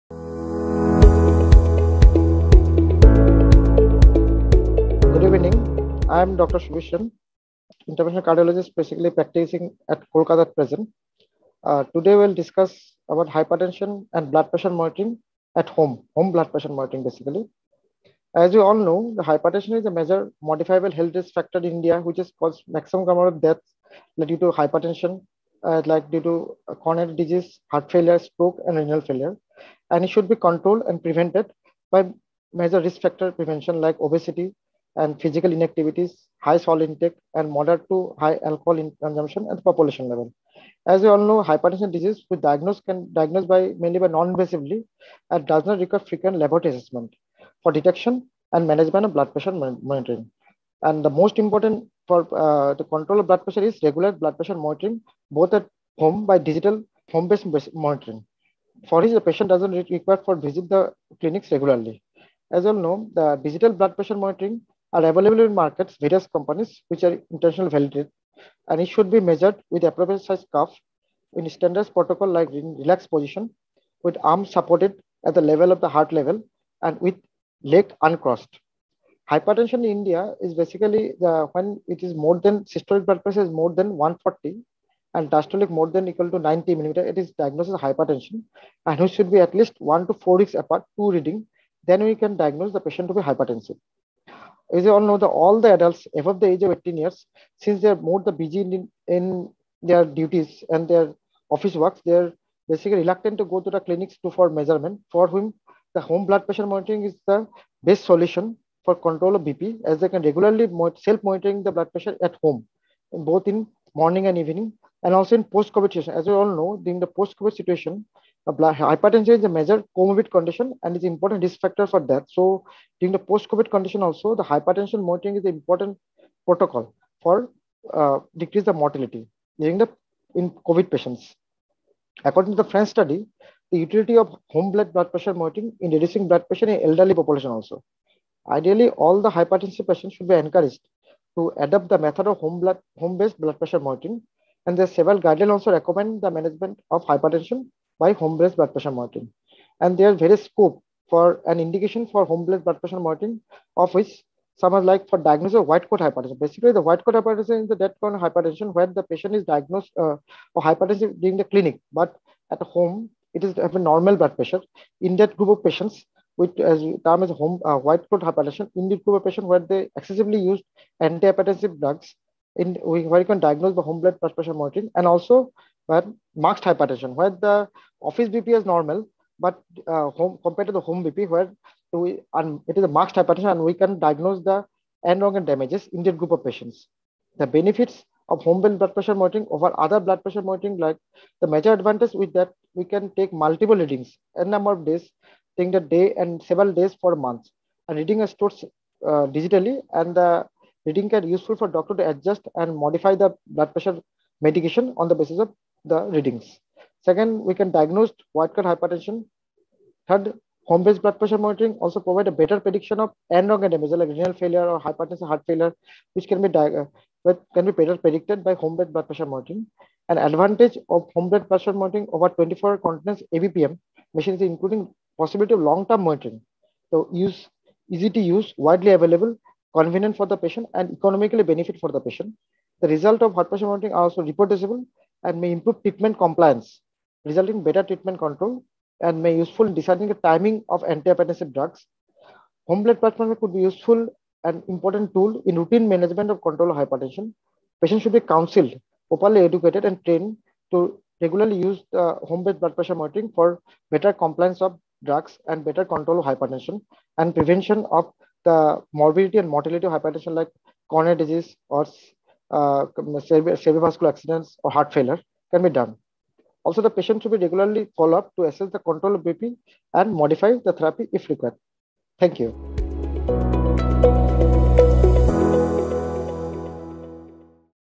Confidence to Achieve BP goal by Dr. Subir Sen
Confidence to Achieve BP Goal

Dr. Subir Sen, DM Cardiologist, SSKM (Kolkata) who is an expert in the field of Cardiology shares his view on “Evolving role of home BP measurement” under the initiative “Confidence to achieve BP goal”
According to a recently published  study, the utility of Home Blood Pressure Monitoring aids in reducing blood pressure in the elderly population. Ideally, all hypertensive patients should be encouraged to adopt the method of HBPM. Several guidelines recommend the use of HBPM for the management of hypertension .